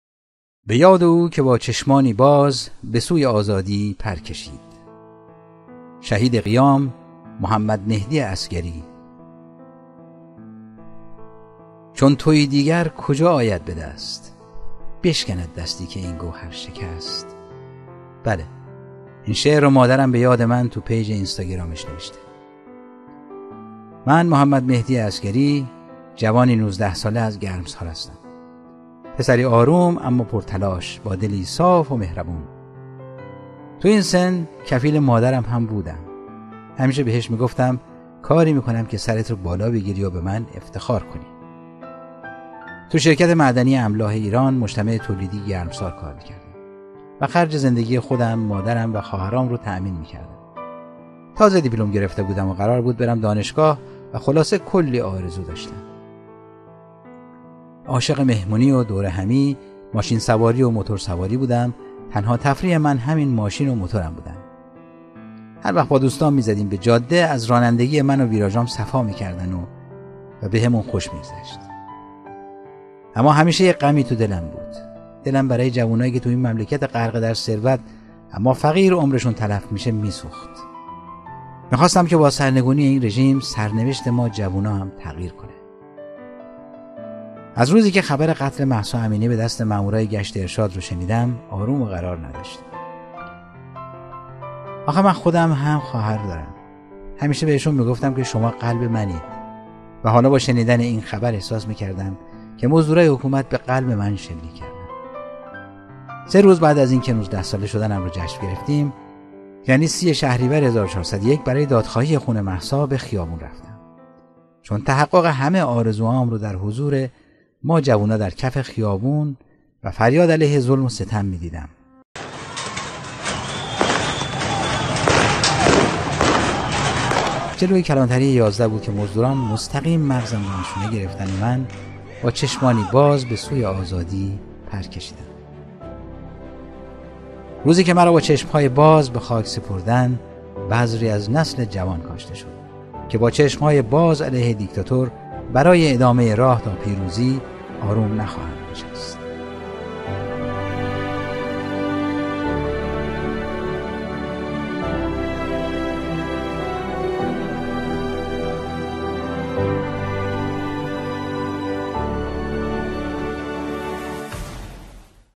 به یاد شهید قیام محمدمهدی عسگری
به یاد او که با چشمانی باز بسوی آزادی پرکشید
سفری می کنیم به حاشیه کویر در استان سمنان و یادی می کنیم از شهید قیام، محمدمهدی عسگری از فرزندان دلیر شهر گرمسار. او که در اوج جوانی، در ۱۹سالگی برای آزادی به خیابون رفت و شب ۳۰شهریور در مقابل کلانتری ۱۱ گرمسار، مزدوران خامنه‌ای با شلیک به مغزش او را شهید کردند.